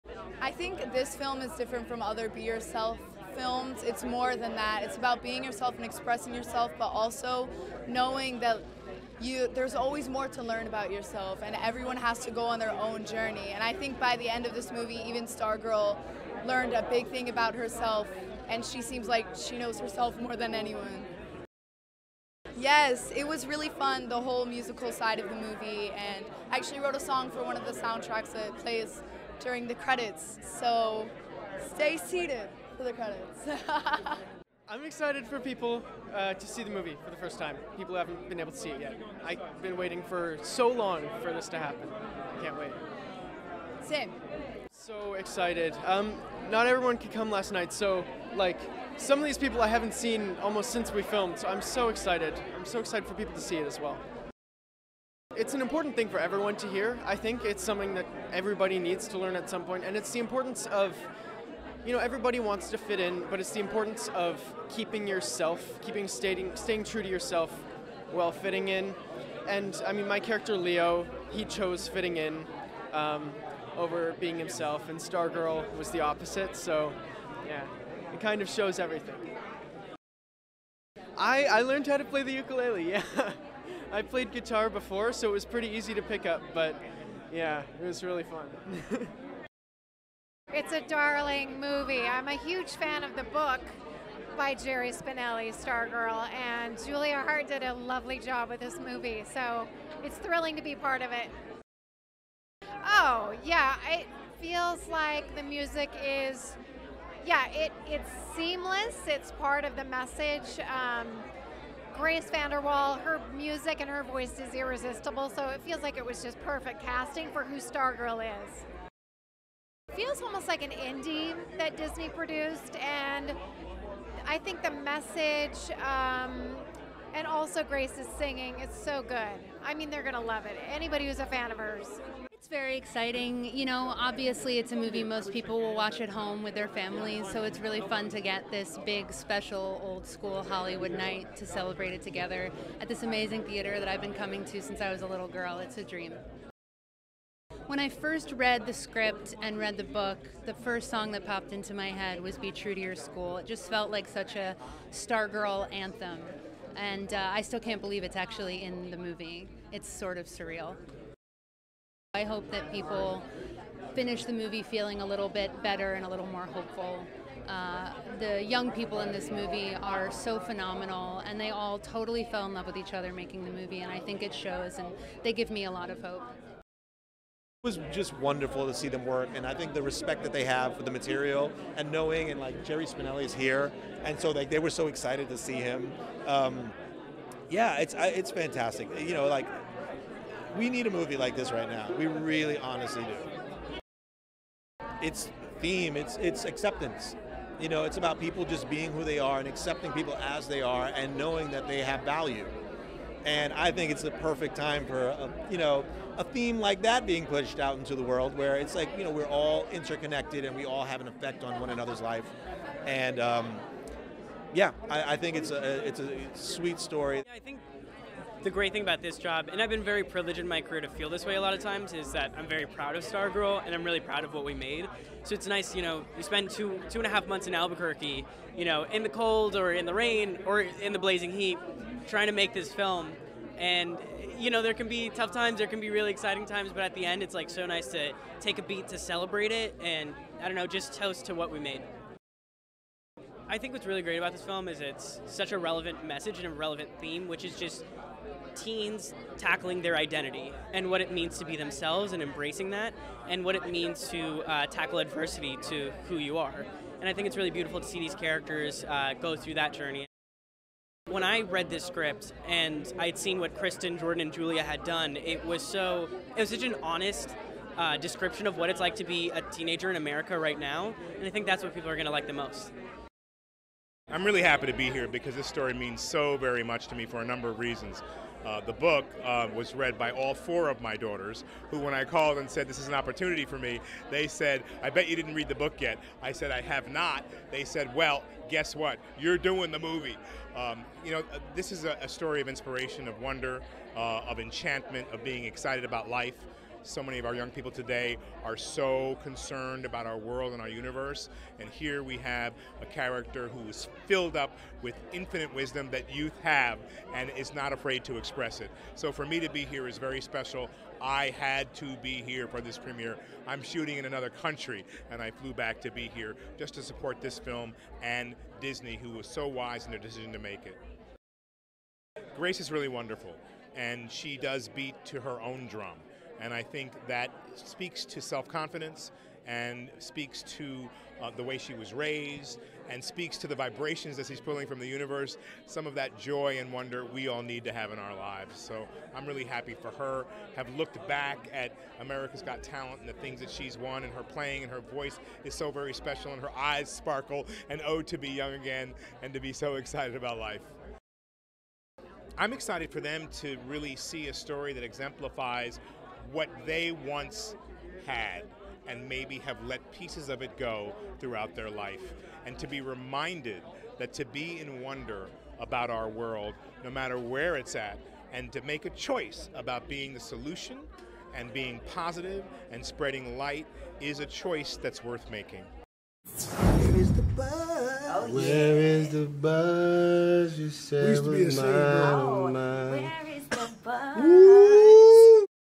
Grace VanderWaal, Karan Brar, Graham Verchere, Darby Stanchfield Talk Disney’s Stargirl
Disney Stargirl is directed by Julia Hart, produced by Kristin Hahn, Ellen Goldsmith-Vein, Lee Stollman and Jordan Horowitz from a screenplay by Hahn, Hart and Horowitz, and stars GraceVanderWaal, GrahamVerchere, GiancarloEsposito, KaranBrar, DarbyStanchfield, and MaximilianoHernández.
The film will be released on March 13, 2020.